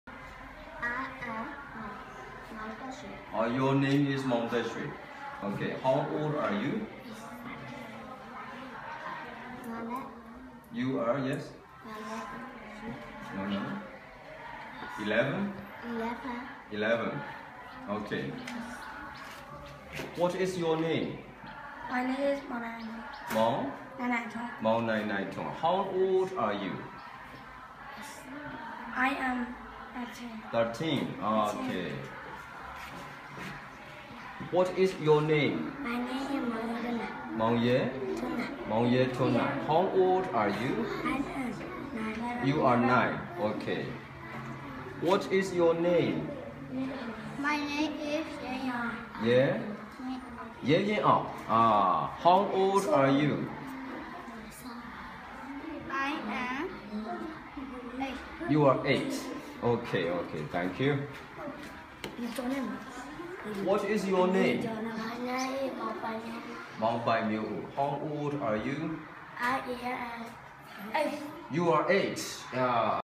Orphans from VWS Myanmar Introduce Themselves
Virtual World School Myanmar aims to connect the poorest children of Myanmar with virtual technology resources.  Here, the orphans of our first project introduce themselves.  Help open up their world and give them the chance to contribute their unique individuality to the human knowledge resource base!  yonearts.org